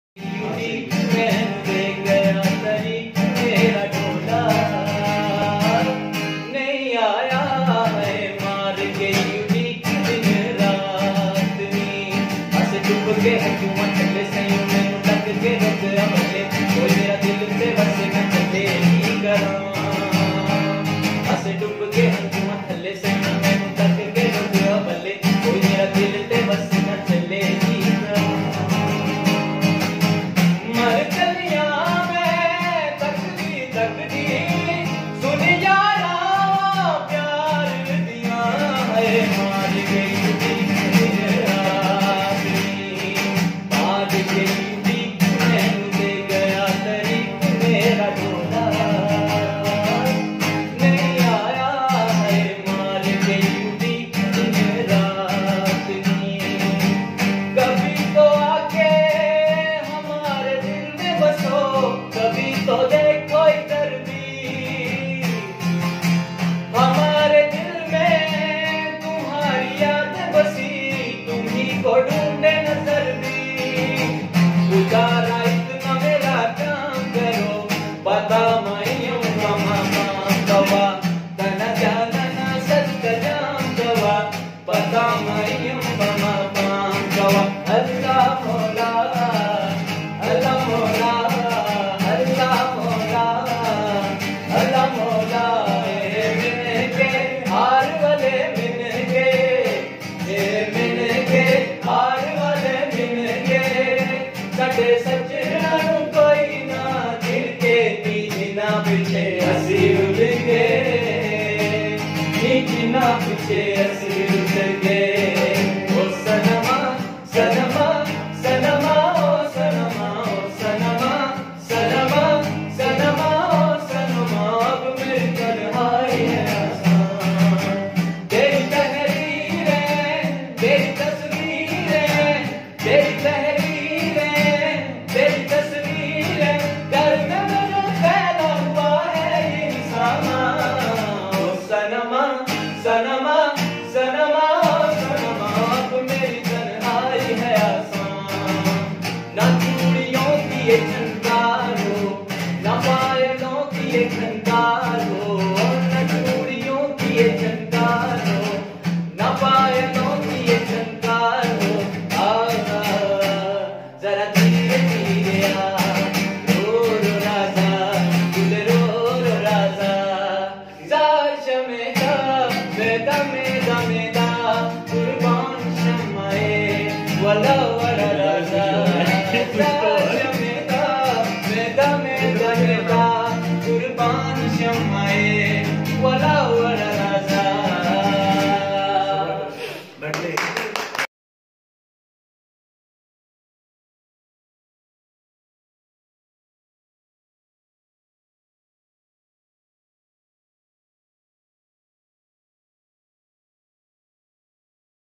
Talented Singer of AJK University | SK Rehman | Mashup
SK Rehman is student of BS Geology in AJK University, Muzaffarabad basically from Rawalpindi. He has singing passion with excellent voice mostly showing his talent at local events but not professionally. Support the talent by SHARING it to others..